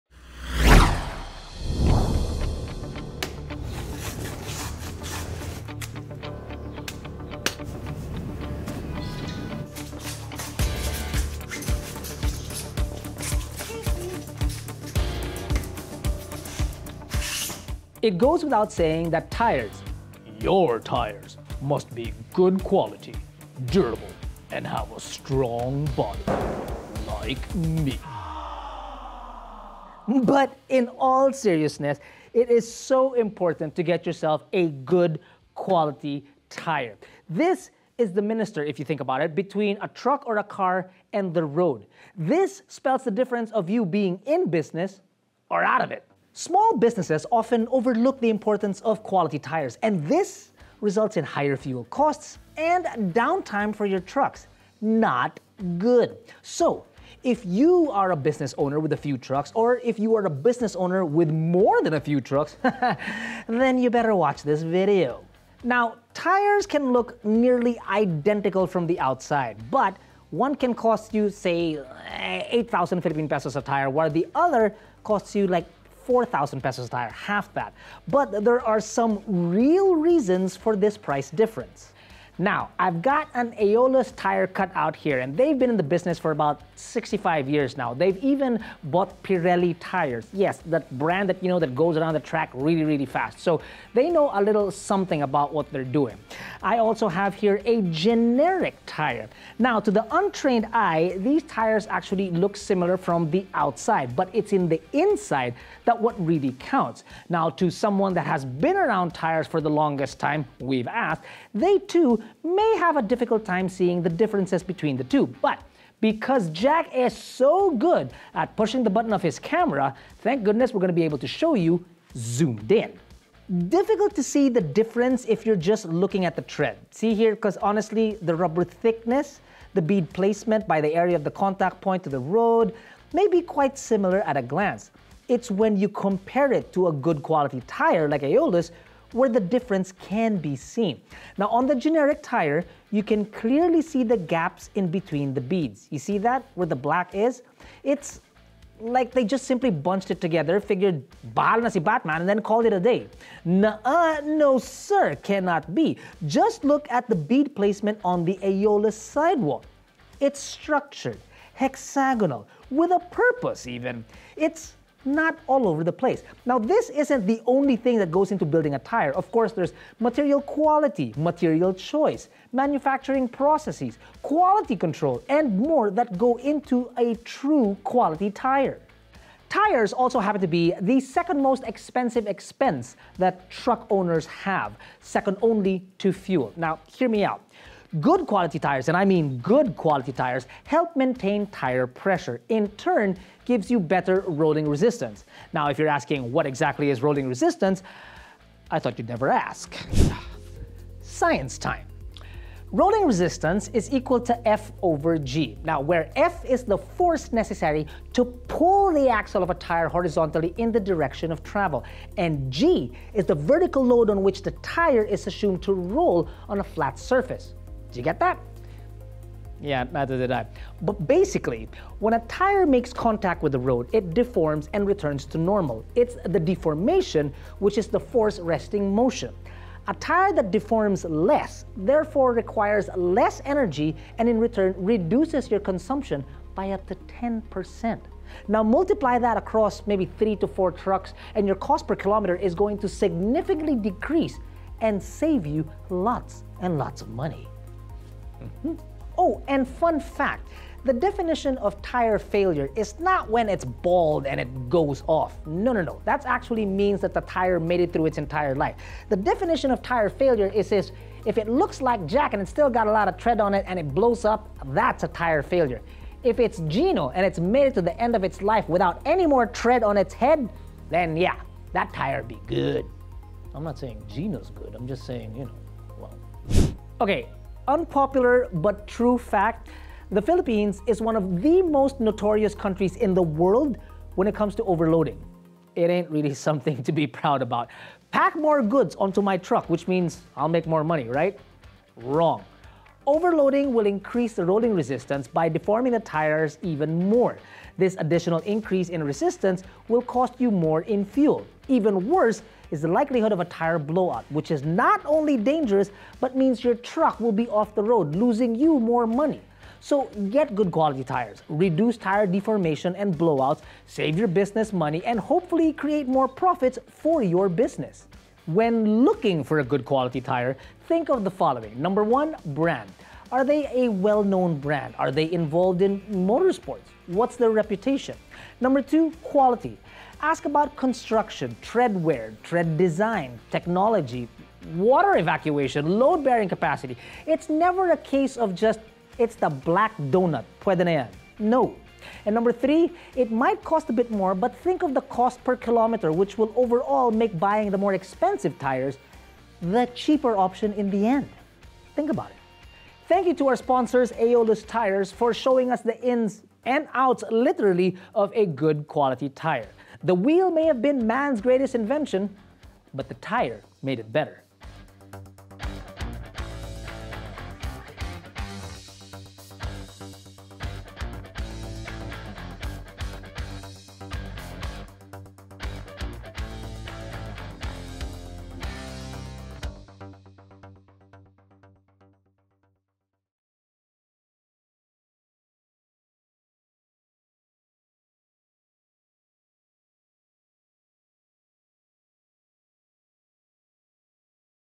How the right tire can save your business money
Going with the cheapest black rubber donut isn't always the best choice. Tires are very important, and they must be up to the task at all times. Quality choices are few and far between in the market, and Aeolus Tyres are up there. With 65 years of tire making experience, and with an emphasis on load-bearing capabilities, they've more than deserved a feature on our channel.